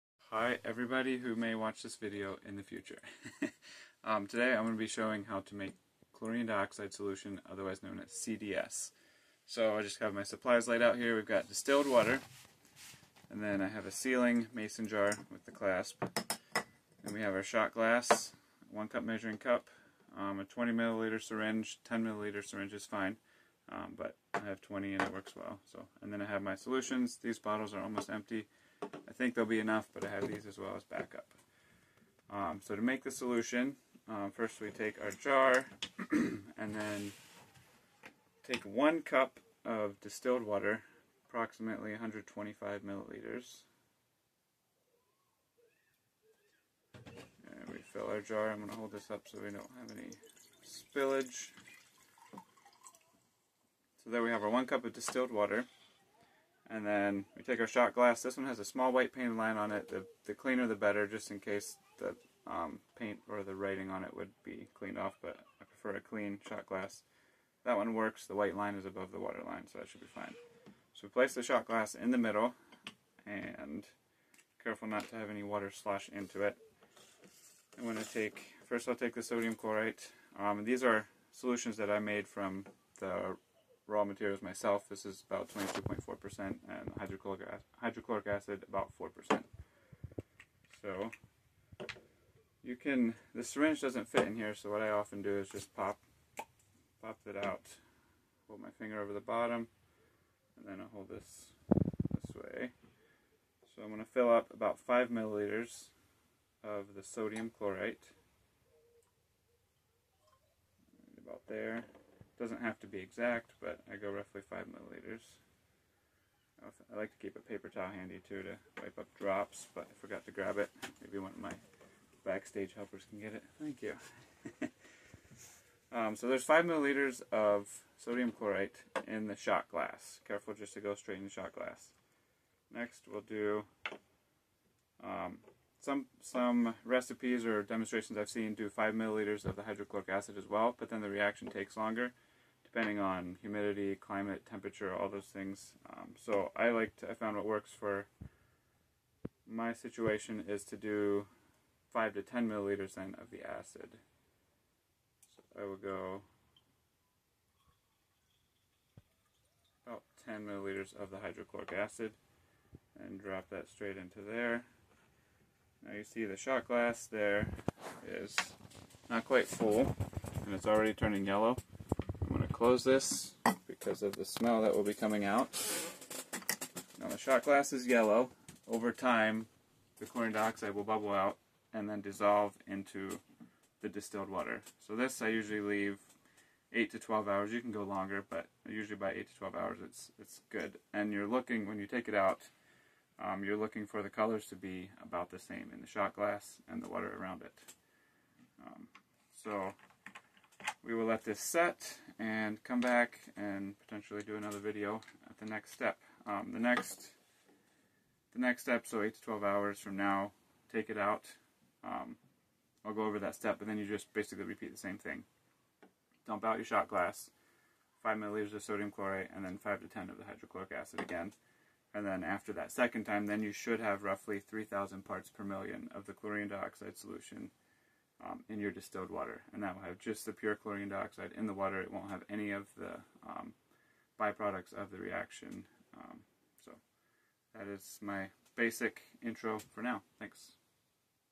How to Make Chlorine Dioxide Solution(CDS) - part 1
Here is part 1 of a quick summary and demonstration of how I make Chlorine Dioxide Solution.
*At 0:42 When describing the amount of distilled water to use I mistakenly said 125mL when I meant 250mL. See this link for my corrected video

This website has several good resources and a well done documentary about Chlorine Dioxide